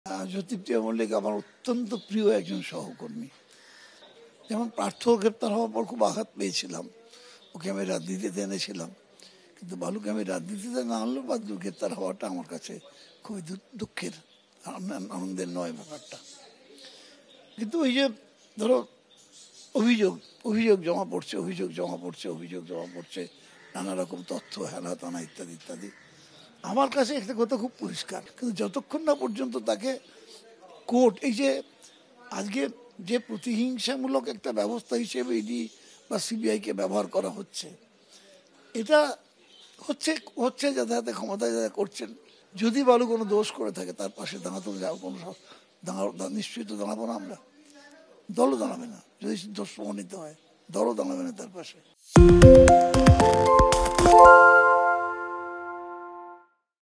Sovandeb: বালু দোষ করে থাকে, তাহলে তিনি পাশে দাঁড়াবেন না, জানিয়ে দিলেন শোভনদেব
ABP Ananda LIVE: বালু দোষ করে থাকে, তাহলে তিনি পাশে দাঁড়াবেন না, জানিয়ে দিলেন শোভনদেব